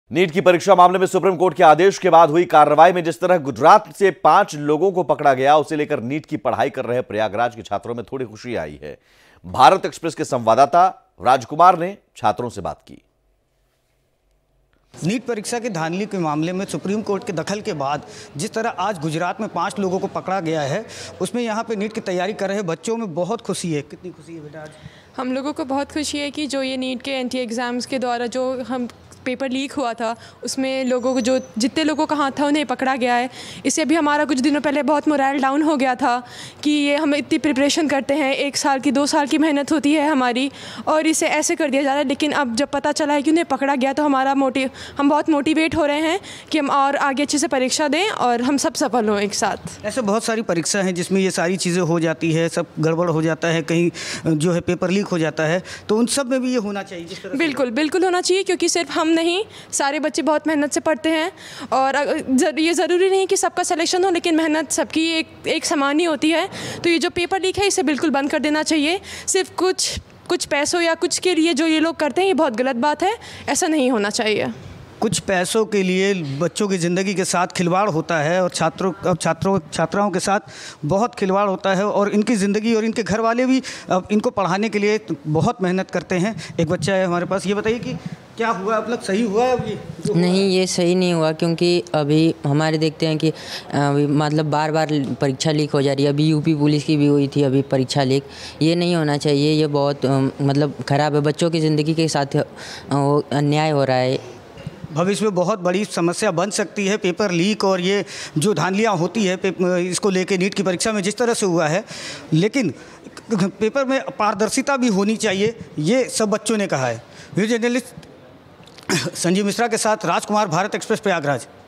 NEET परीक्षा मामले में SC का कड़ा एक्शन, 5 लोगों की गिरफ्तारी! खुश हुए NEET की तैयारी कर रहे छात्र
Thanks & Regards
Bharat Express